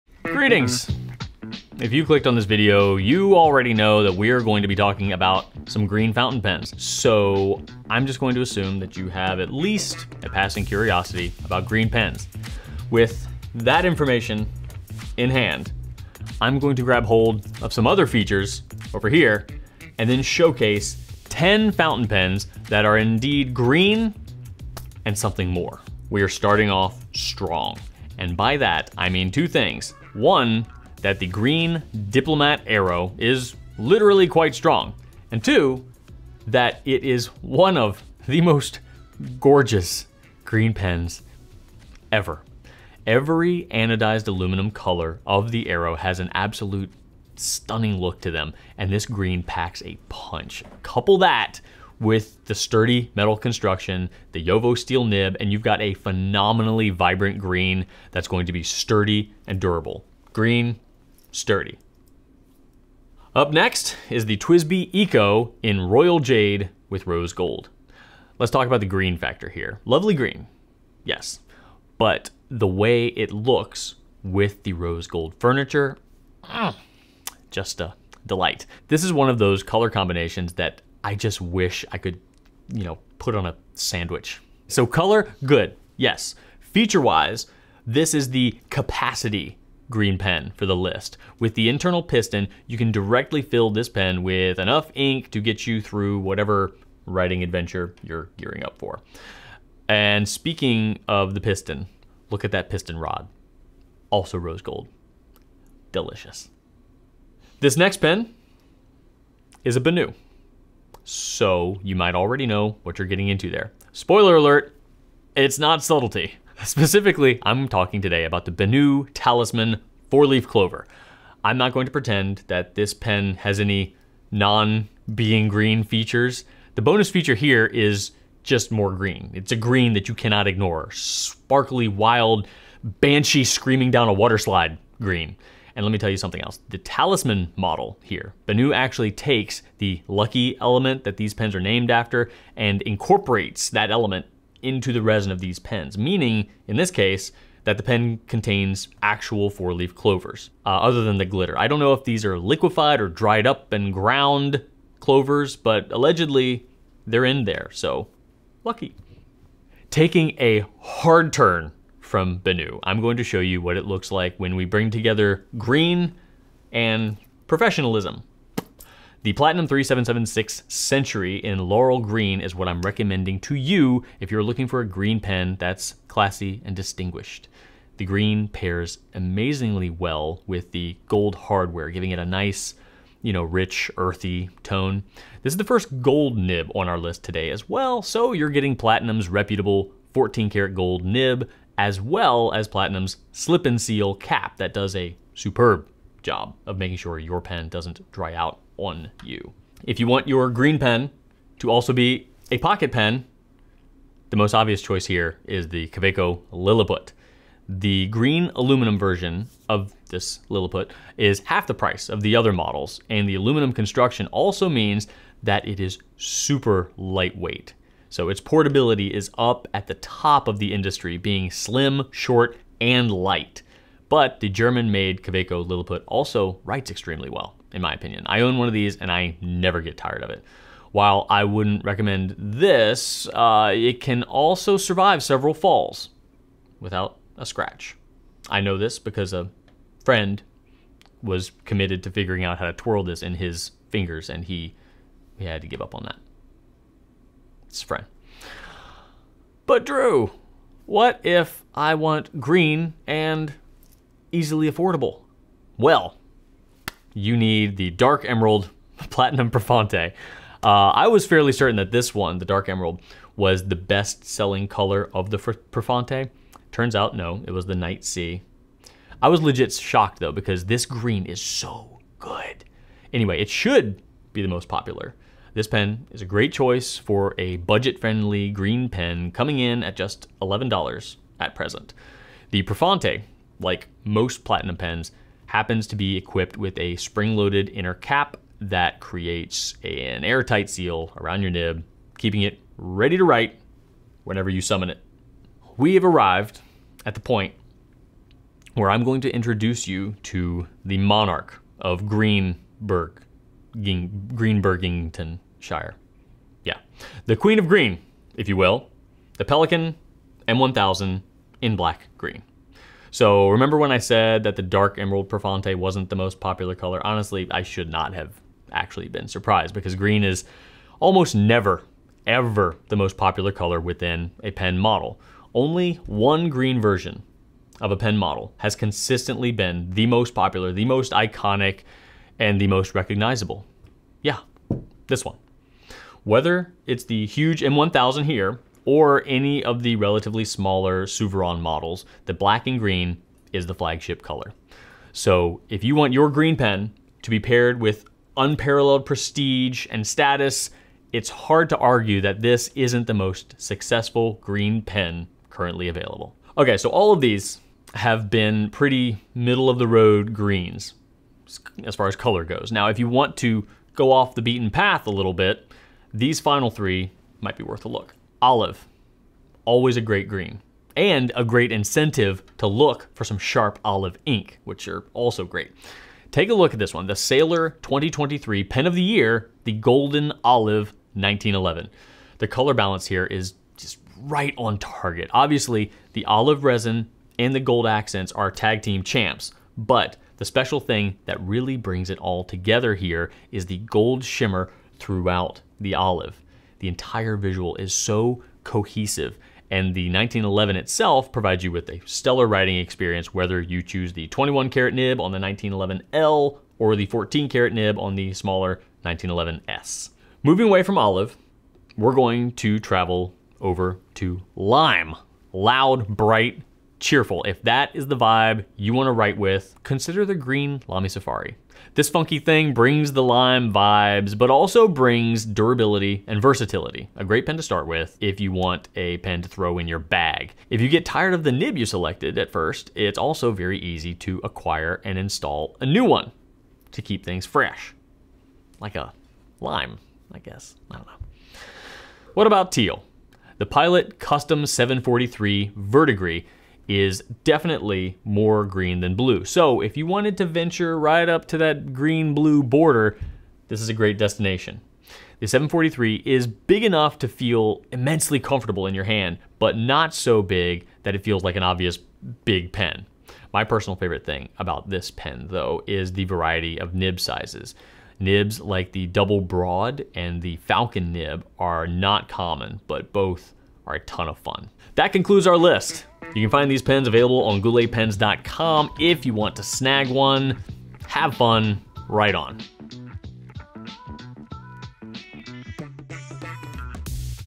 10 Awesome GREEN Fountain Pens!
So, you're interested in a green pen? Well, what other features are important to you? Drew's here to help you identify which green fountain pen has the right super powers for your writing adventures!

TIMESTAMPS:
0:00 Intro
0:25 Diplomat Aero
1:02 TWSBI Eco
1:47 BENU Talisman
2:40 Platinum 3776
3:20 Kaweco Liliput
4:10 Platinum Prefounte
4:56 Pelikan M1000
6:08 Sailor 1911
6:50 LAMY safari
7:22 Pilot Custom 743